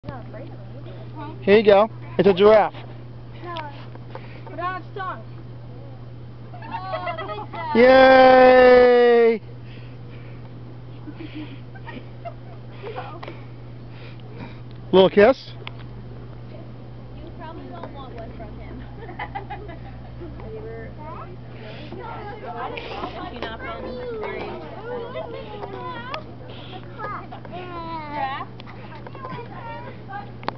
Winter Skyy Feerer at the Cincinnati Zoo, Giraffee pt1
Winter Skyy Feerer tried feeding the giraffes rye crackers